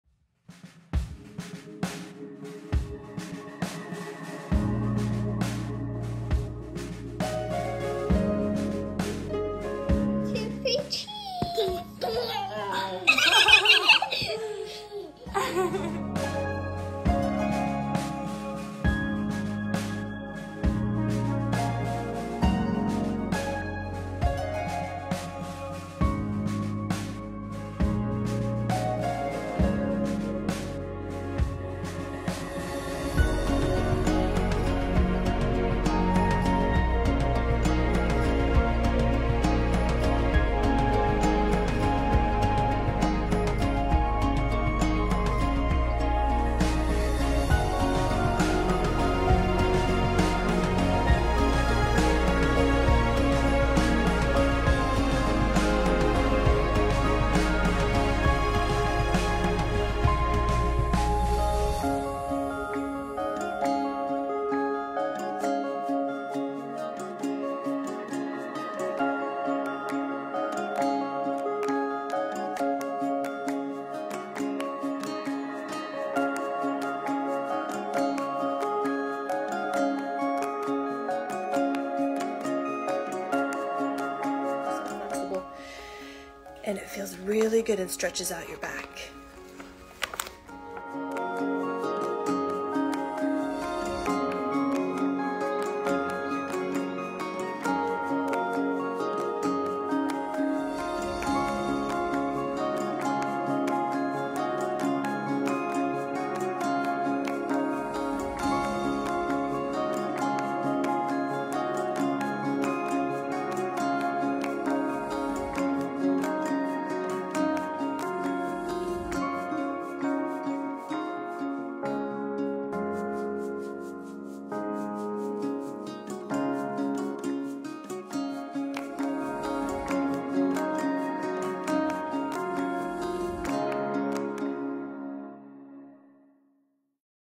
Yoga with Serena intro
I’m Serena-a wife, mother, and yoga teacher, meditation teacher, and sound healer. Let’s see what’s possible!